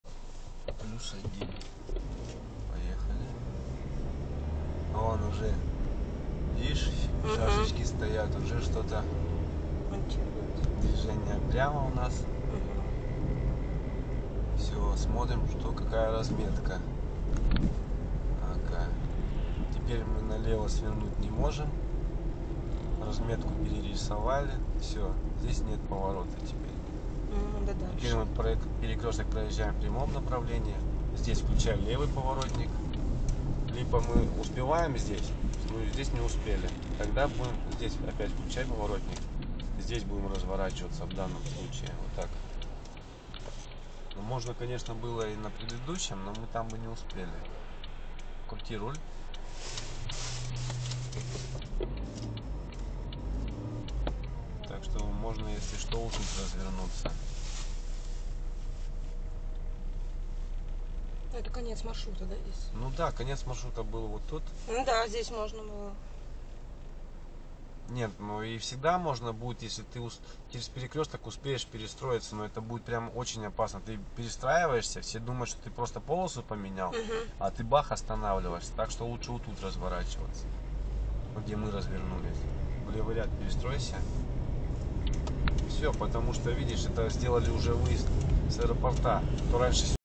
Район аэропорта
Поддержите подпиской) буду благодарен. Задавайте вопросы, отвечу всем. автоинструктор27 на момент создания ролика, на этом перекрестке нанесли границы движения в полосе